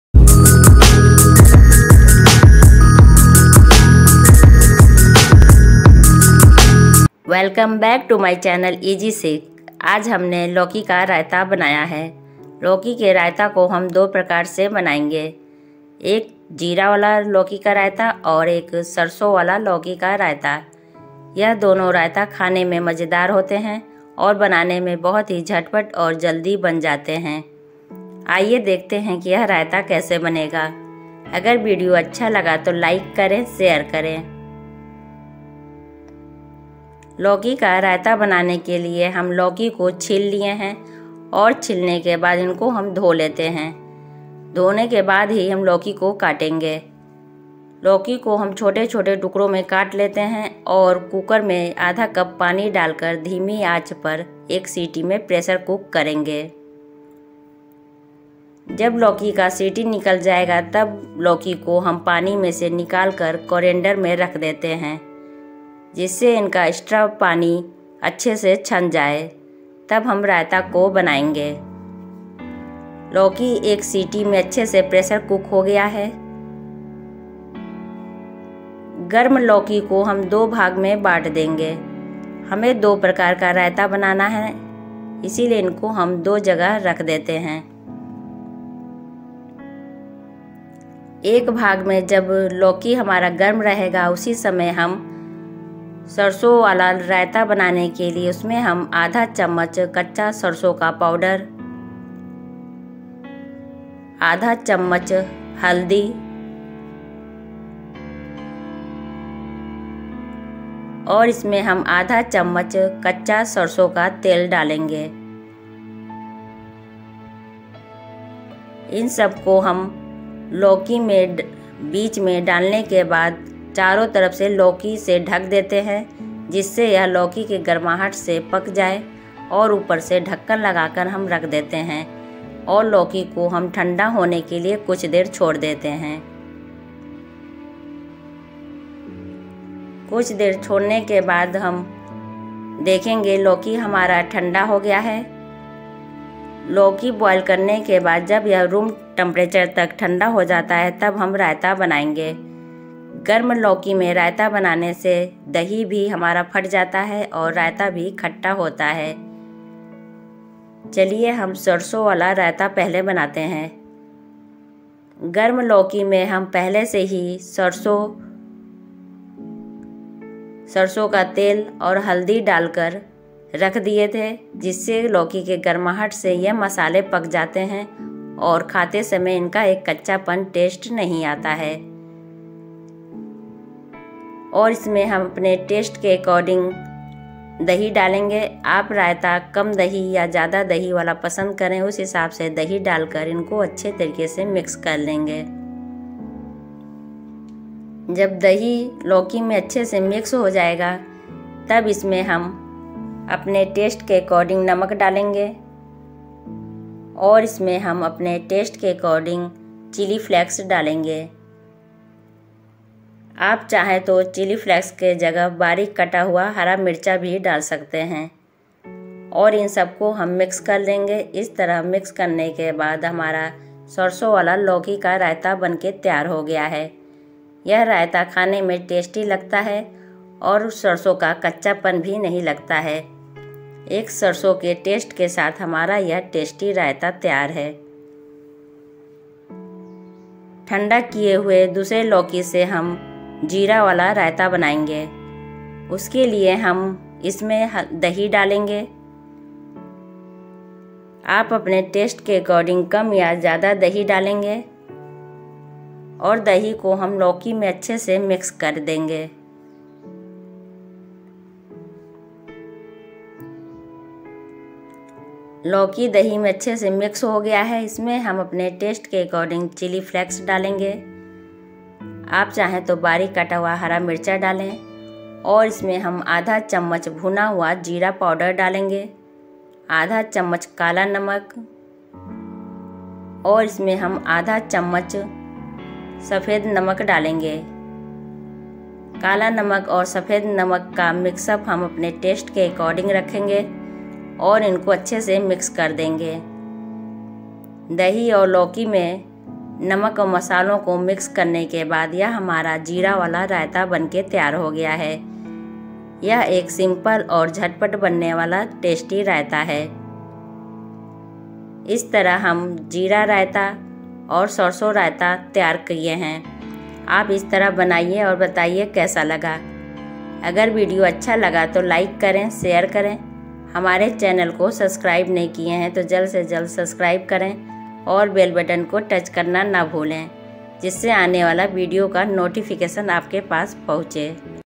lauki ka raita | लौकी का जीरा और सरसो वाला tasty & healthy रायता बनाने का आसान तरीका
thanks 🙏🙏